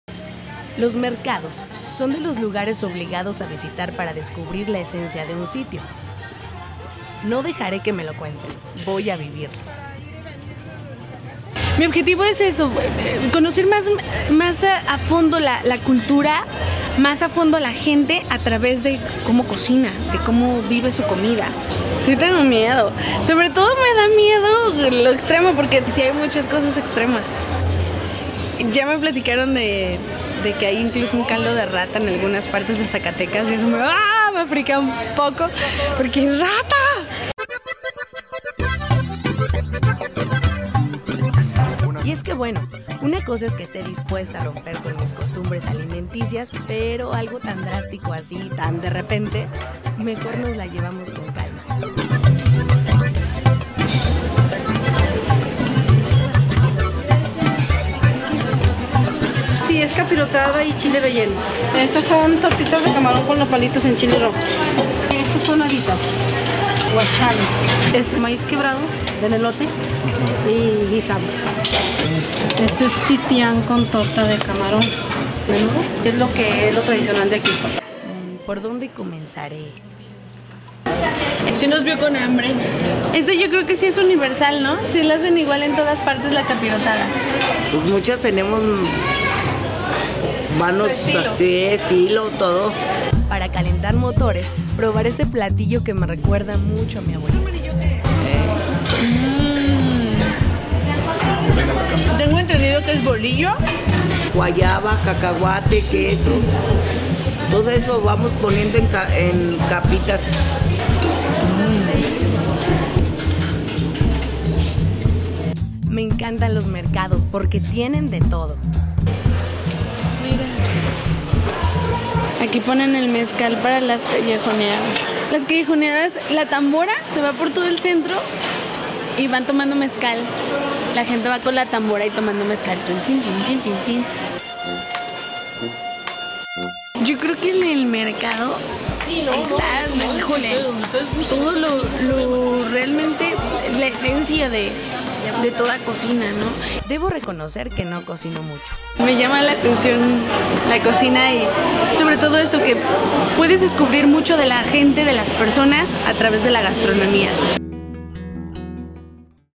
El mercado de Jerez, Zacatecas
No hay mejor lugar para adentrarse a la gastronomía de un sitio que el mercado. Es por ello que Raquel decide comenzar ahí su aventura gastronómica.